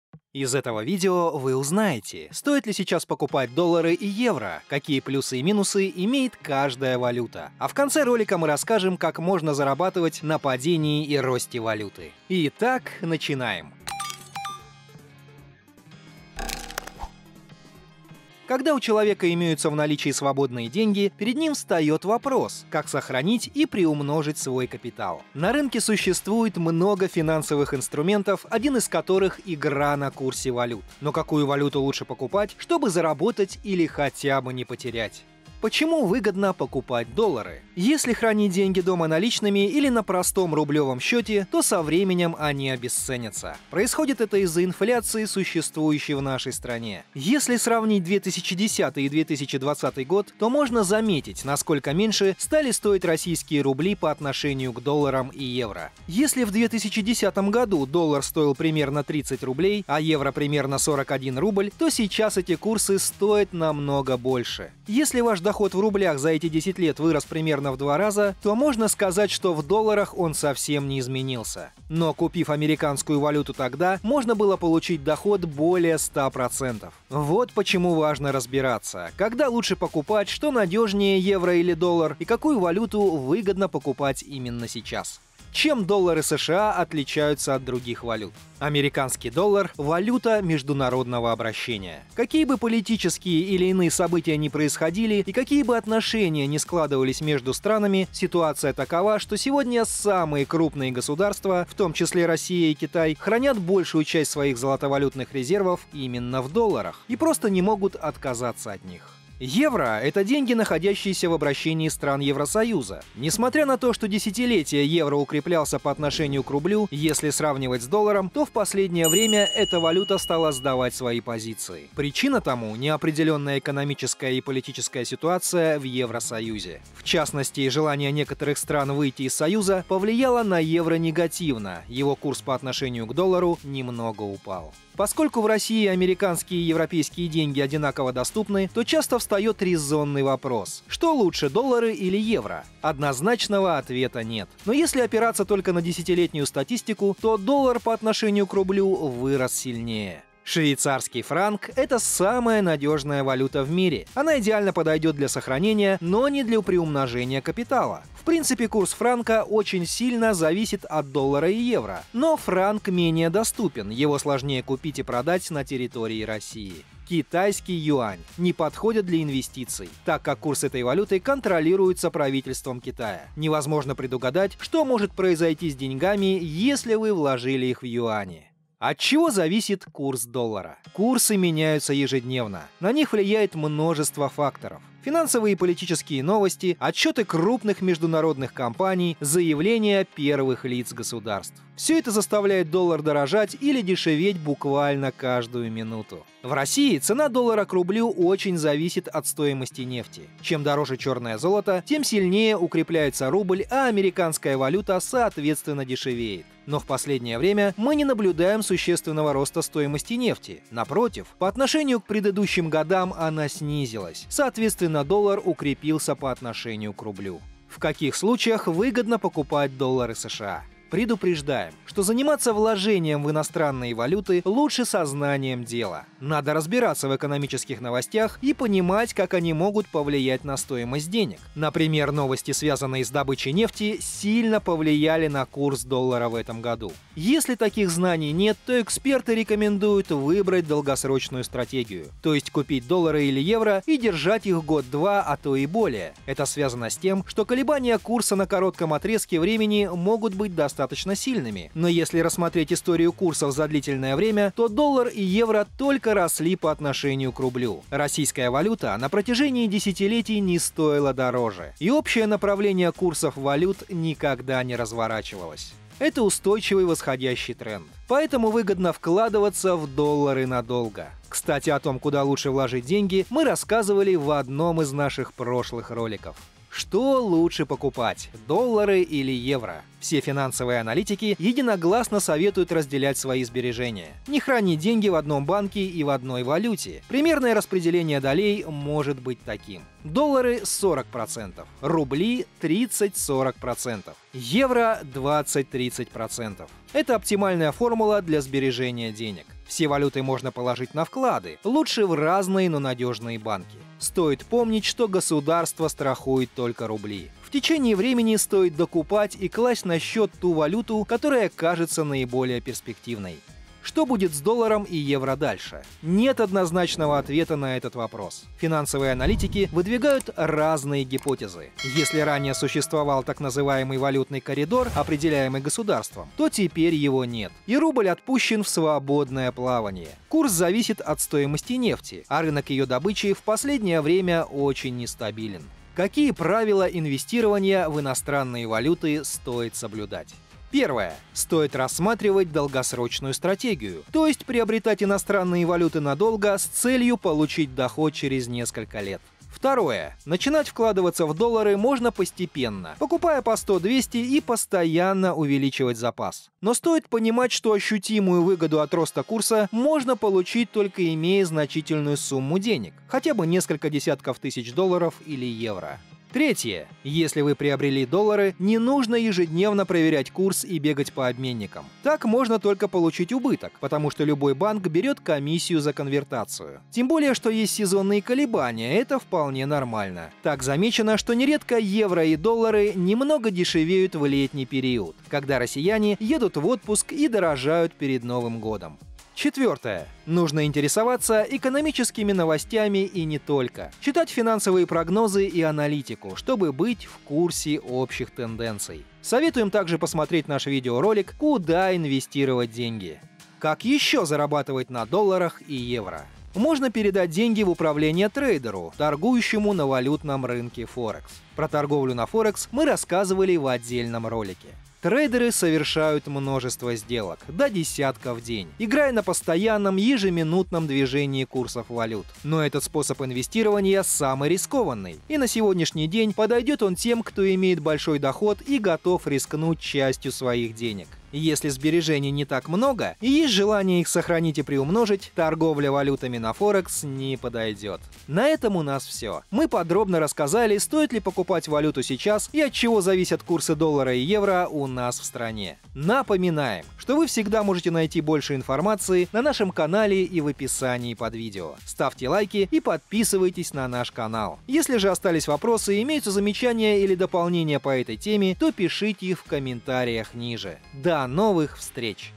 Стоит ли покупать доллары и евро сейчас? Причины, почему стоит брать валюту в 2025 году
🚀 Покупаю акции и криптовалюту на этой Бирже В этом видео расскажем, стоит ли покупать доллары и евро сейчас. И вообще стоит ли покупать валюту сейчас или можно повременить. Итак начинаем.

Содержание:
Вступление 00:00
Почему выгодно покупать валюту и стоит ли покупать доллары и евро сейчас 00:36
Чем доллары США отличаются от Евро и других валют 01:25
От чего зависит курс доллара 02:59
В каких случаях выгодно покупать доллары США 03:38
Что лучше покупать, доллары или евро 04:37
Что будет с долларом и евро дальше 05:14
Правила инвестирования в иностранные валюты 05:35
Как зарабатывать на долларах и евро 06:43

Мы рассмотрели основные вопросы - стоит ли покупать доллары и евро (валюту) сейчас или можно повременить, какие перспективы у валюты по отношению к рублю и так далее.

Оставляйте вопросы по теме, а также свои замечания и пожелания в комментариях под этим видео.

Предупреждение о рисках: торговля финансовыми инструментами является рискованной деятельностью и может приносить не только прибыль, но и повлечь убытки.